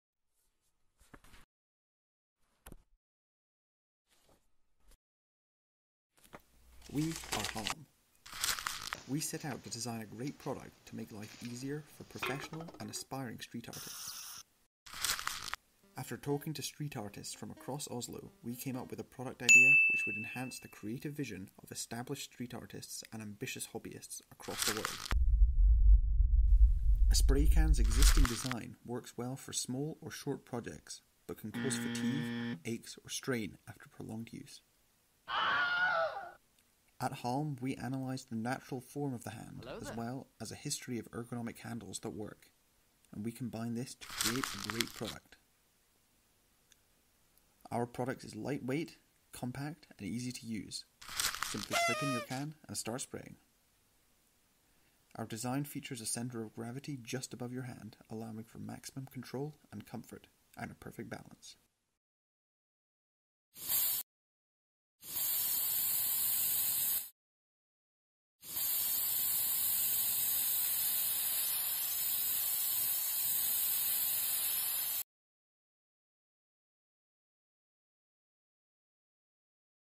Spray Painting Grip
A stop motion animation for a spray painting grip made while on Erasmus exchange to Oslomet University in Norway